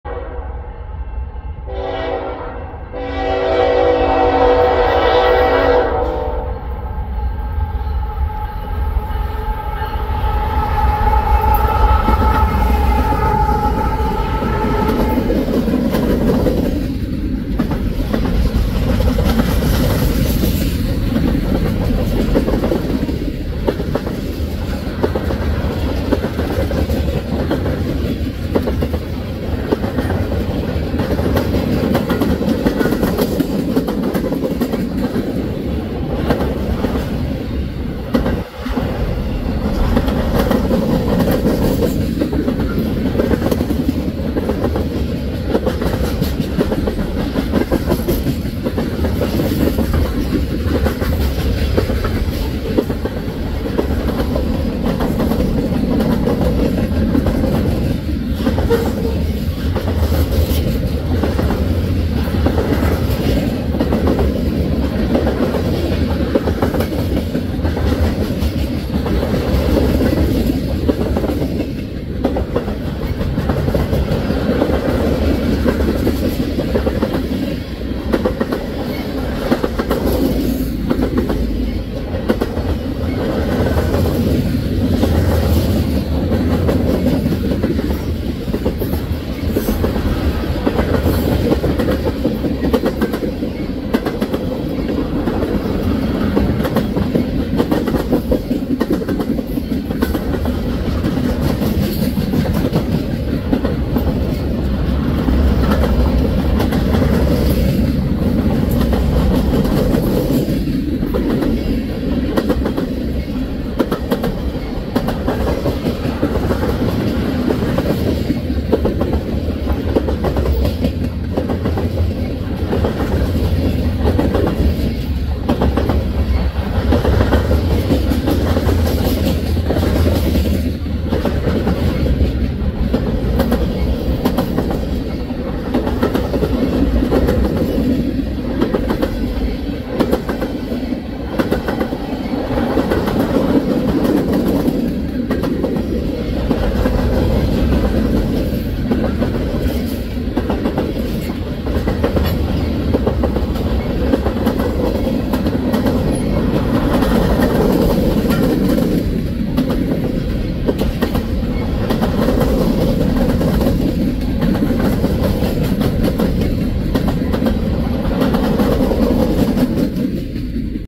Canadian National - Eastbound Freight 5/10/24
Lansing, MI  -  Washington Avenue crossing at 1218 EDT

CN ES44AC 3878 and 3957

Broken cloud cover makes outdoor video making interesting. A fixed aperture complicates things.